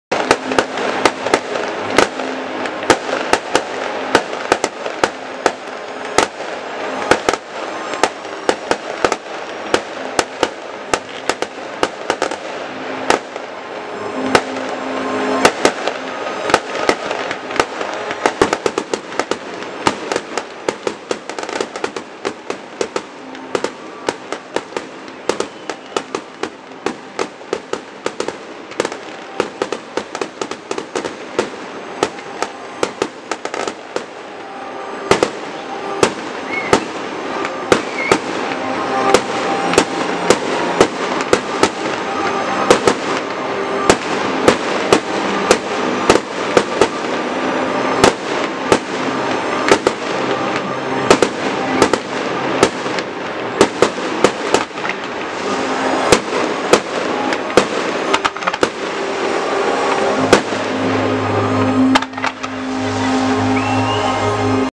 Havířov v květech - ohňostroj
V centru Havířova se v sobotu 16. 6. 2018 konala tradiční slavnost Havířov v květech.